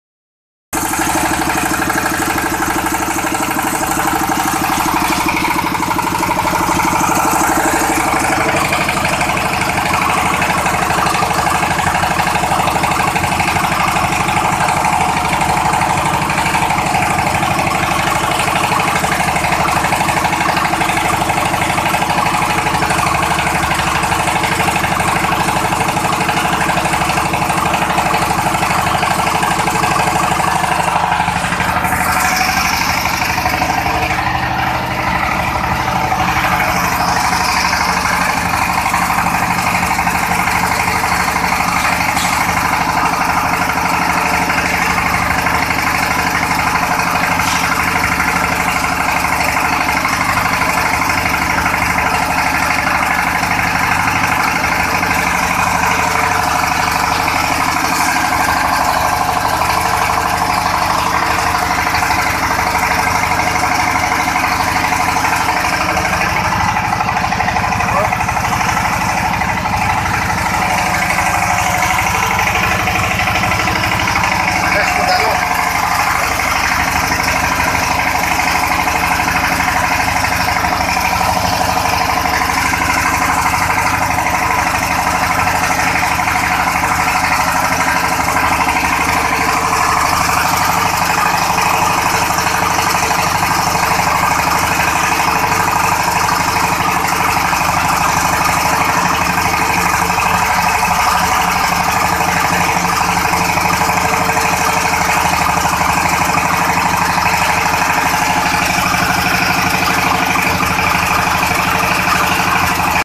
Mesin press limbah plastik penggerak diesel
Mesin press limbah botol plastik penggerak diesel 16hp info hub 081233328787 atau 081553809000